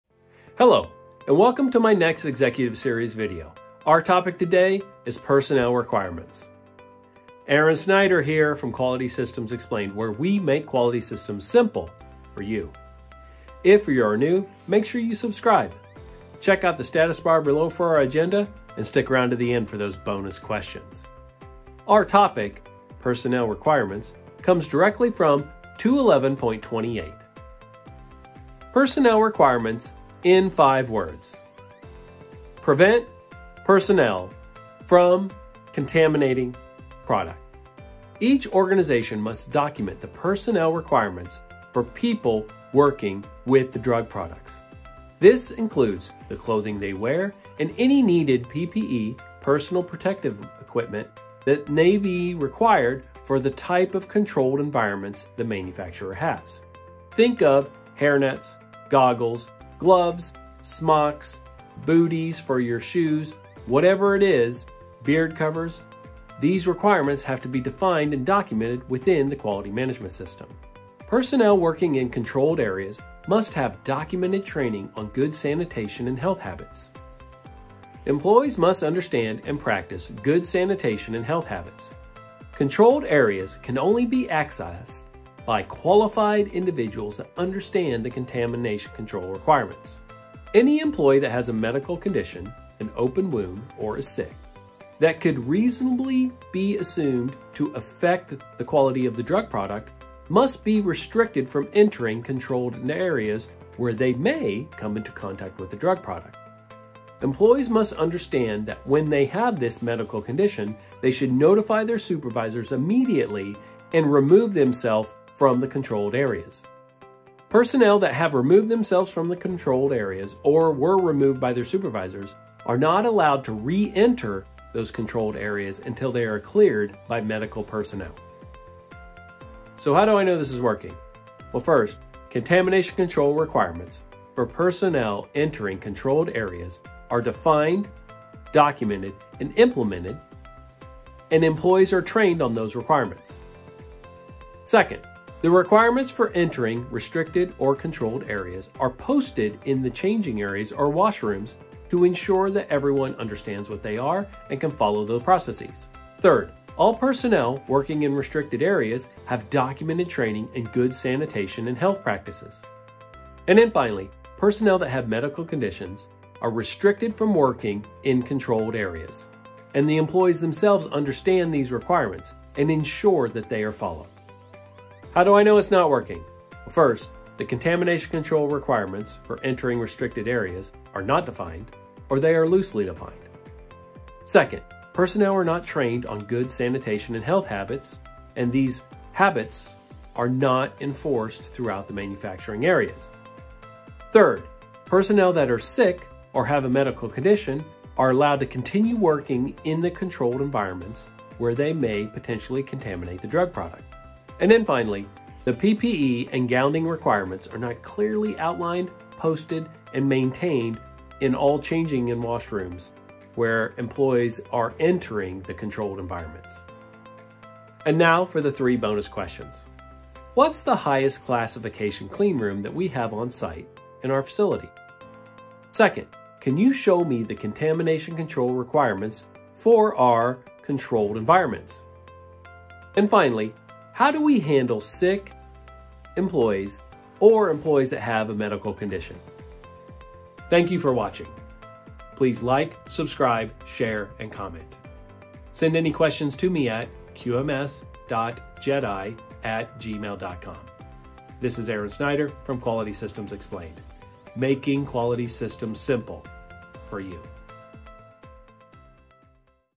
Personnel Responsibilities § 211.28 (Pharma Executive Series #13)
Our requirement, Personnel Responsibilities, comes directly from § 211.28

Personnel Responsibilities in 5 words:
Prevent Personnel from Contaminating Product